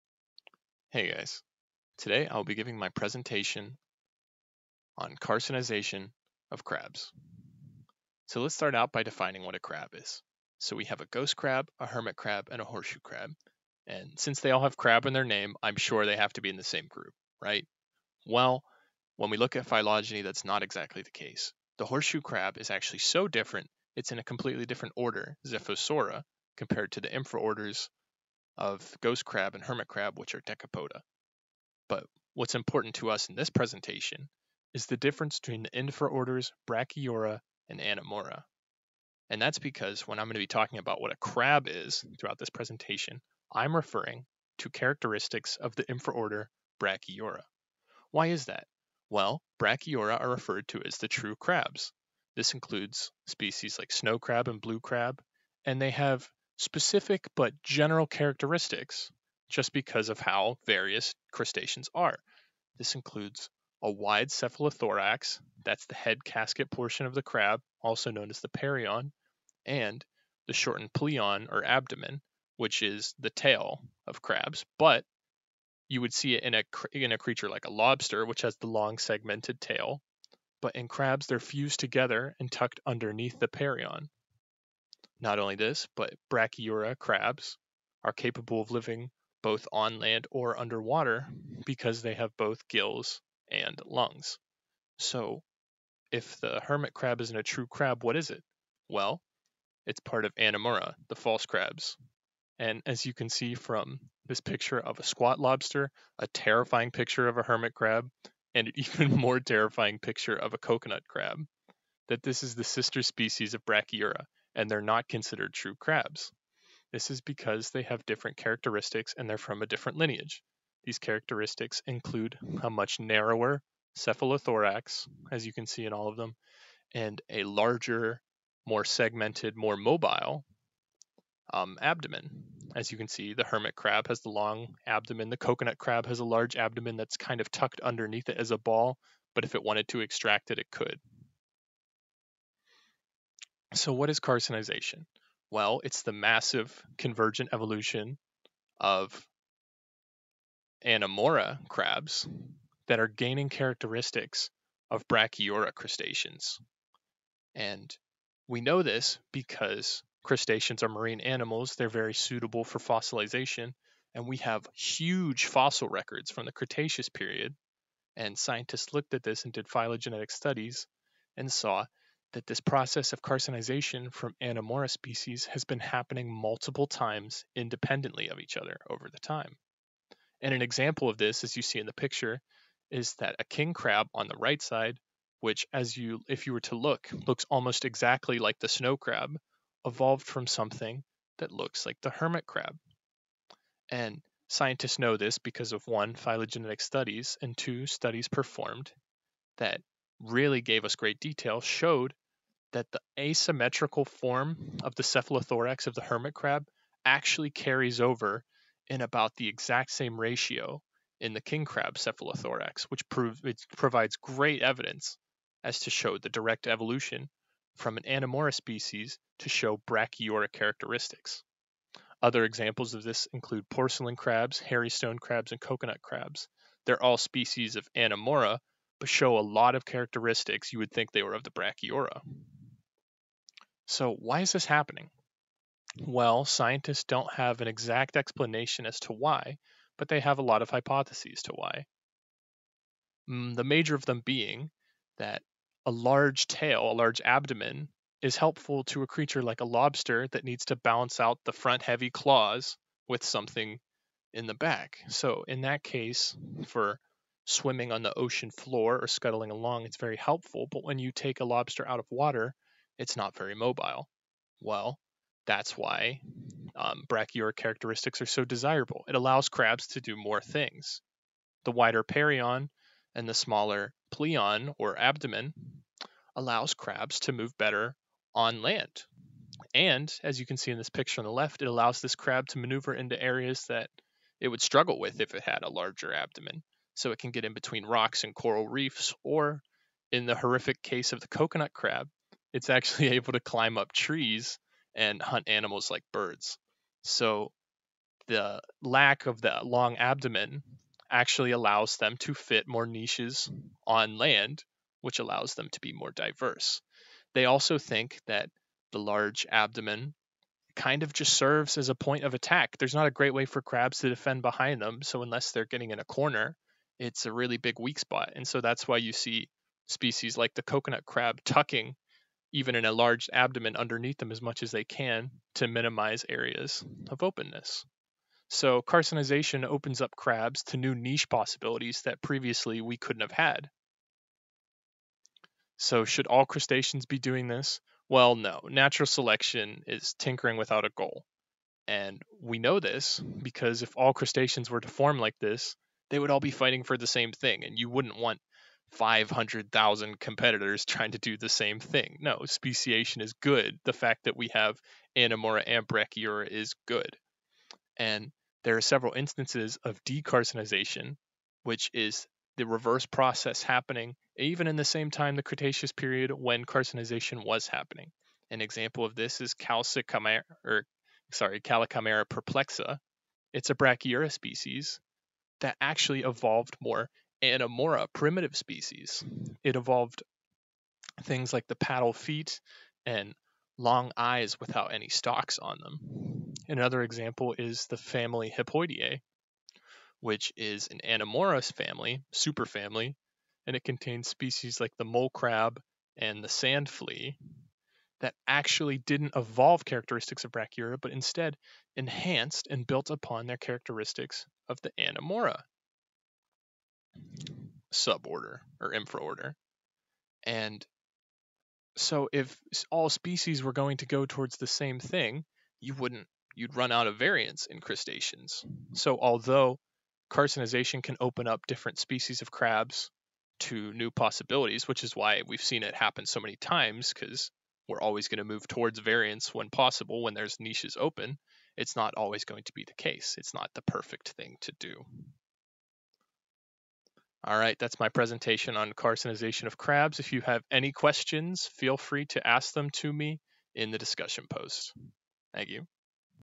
Carcinization of Crustaceans - Christopher Montgomery
The Subphylum Crustacea contains a wide array of invertebrates, including a large number of species of what most people consider "crabs".  In the scientific world "crabs" only of those species within the Infraorder Brachyura, which means True Crabs.  Even though this Infraorder contains all of the True Crabs, scientists have found that multiple other crustacean species in different Infraorders have developed characteristics near identical of those in Brachyura.  This process of non-Brachyura species developing characteristics of these True Crabs is called Carcinization, and has been found to most commonly occur within it's sister group, Anomura.  Anomura contains the False Crabs, where most species have characteristics similar to lobsters and shrimp.  On several occasions independent to each other, Anomura species have been found to evolve characteristics near identical to True Crabs, raising the question as to why these characteristics are so favorable in the marine world.  Scientists have hypothesized that these Convergent Evolution events allow for access to new terrestrial niches for crustaceans. Contrary to this, evidence of Decarcinization, the loss of True Crab characteristics, was observed in the same time period that an explosion of Carcinization was occurring.